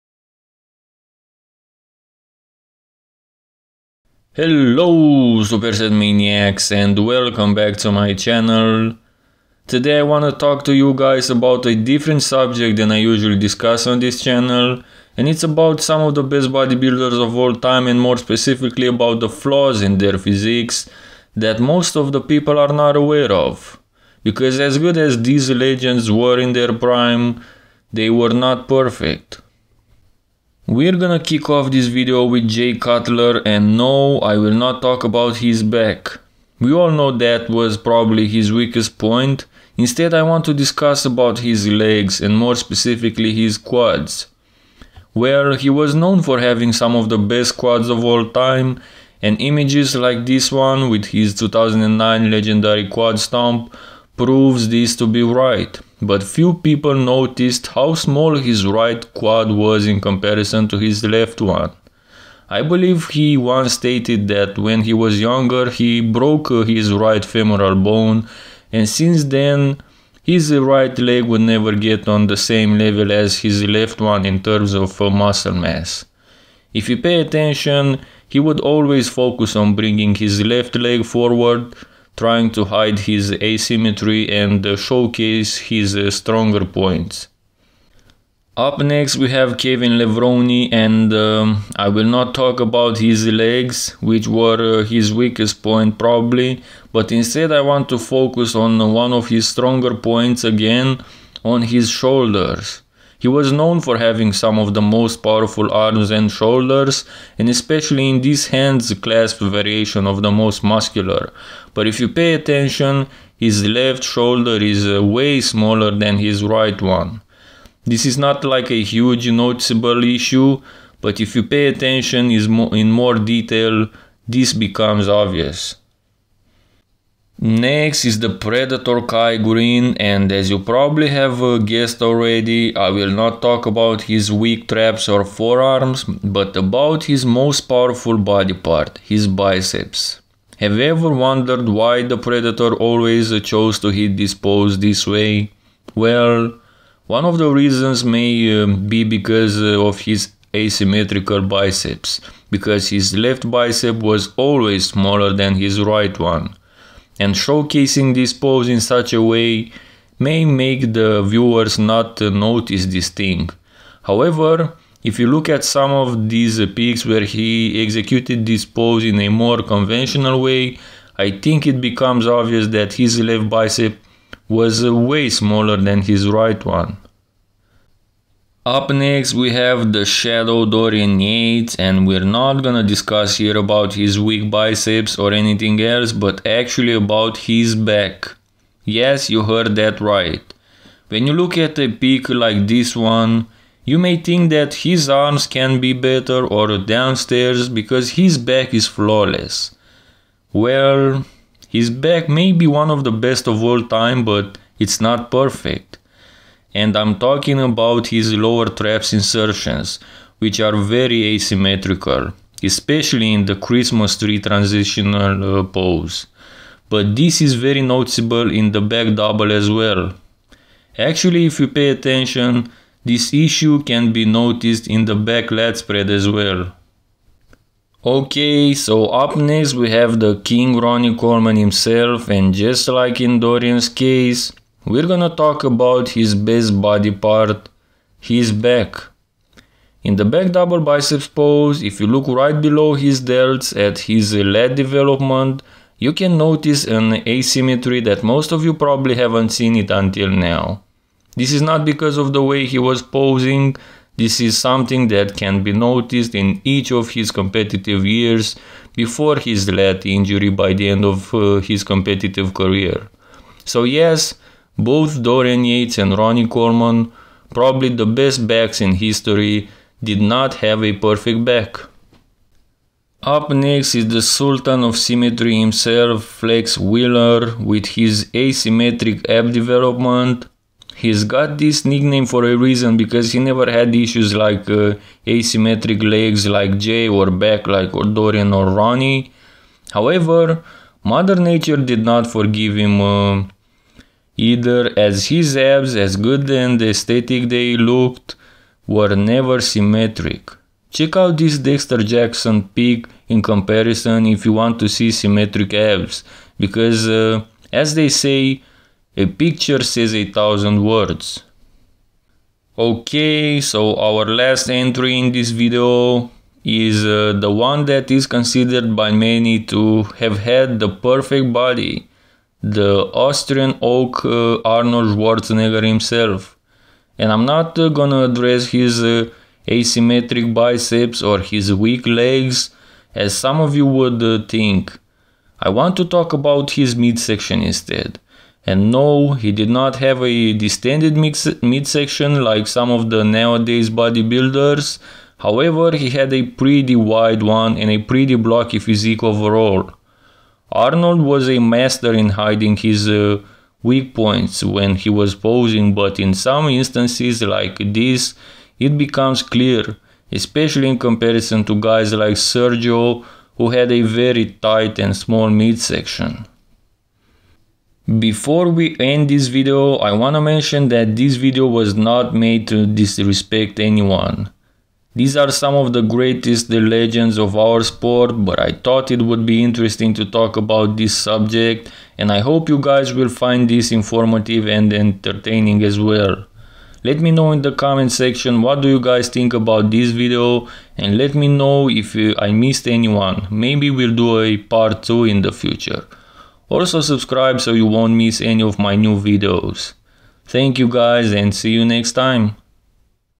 Flaws You Never Noticed On Your Favorite Bodybuilders
Arnold Schwarzenegger, Ronnie Coleman, Jay Cutler, Dorian Yates, Flex Wheeler, Kevin Levrone, Kai Greene and more.